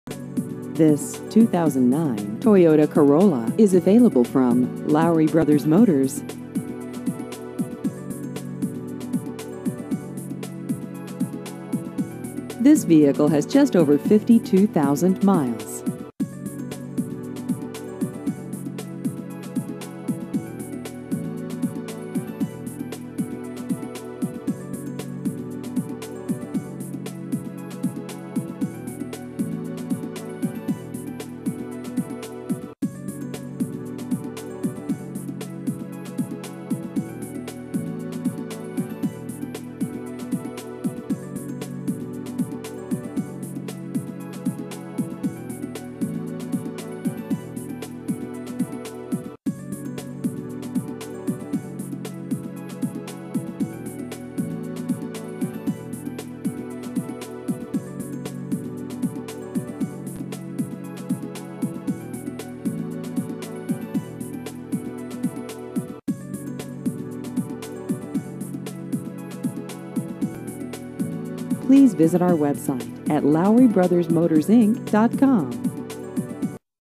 2009 Toyota Corolla Used Cars Boaz AL
This 2009 Toyota Corolla is available from Lowery Brothers Motors.